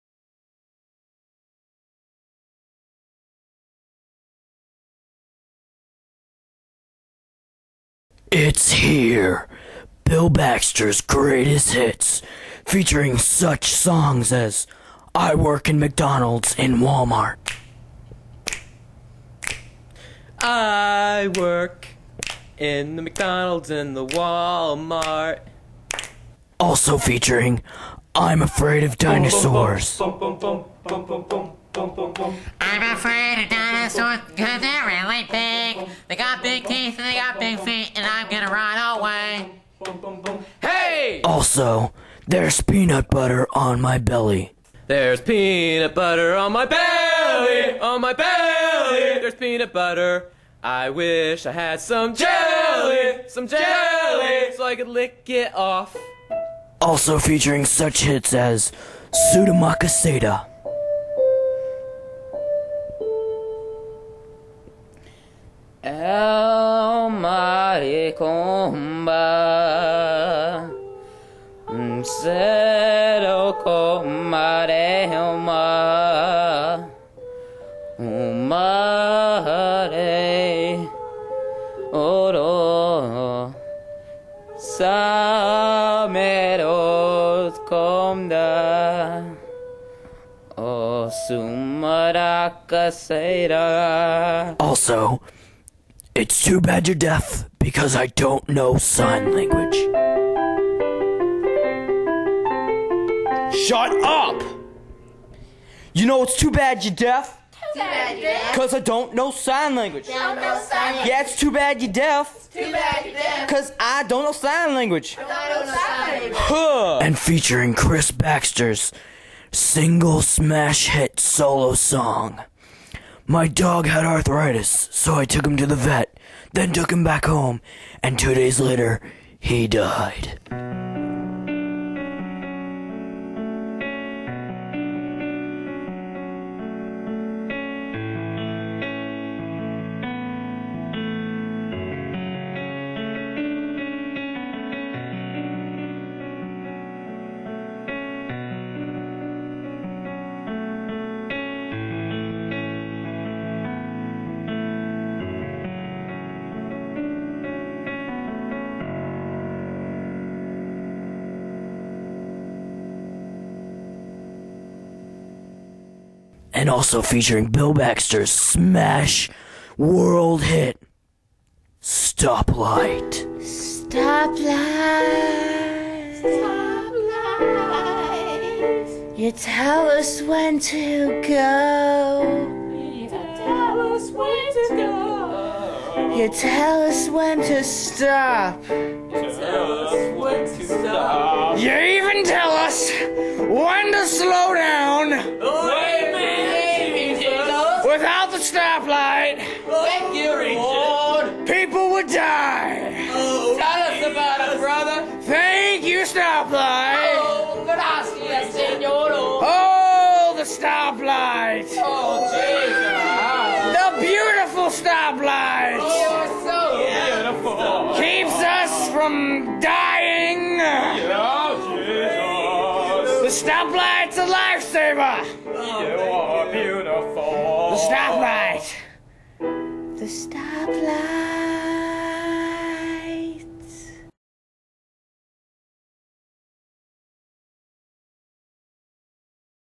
Bill Baxter - Season 2 - Greatest Hits
Bill Baxter's greatest hits. Some pretty darn good singing in there if you ask me.